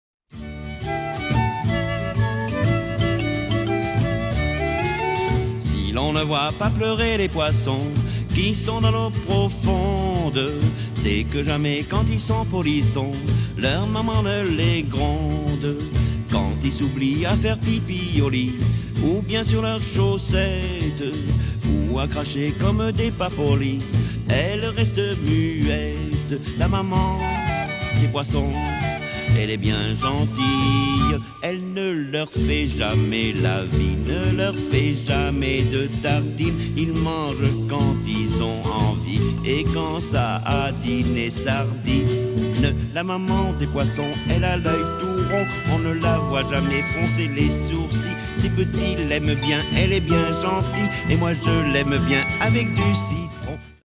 Expo photo La Loupe 2010
Les photos de Sébastien Hervé sur une musique de Boby Lapointe
Au château de La Loupe (28240) jusqu'au 10 octobre 2010.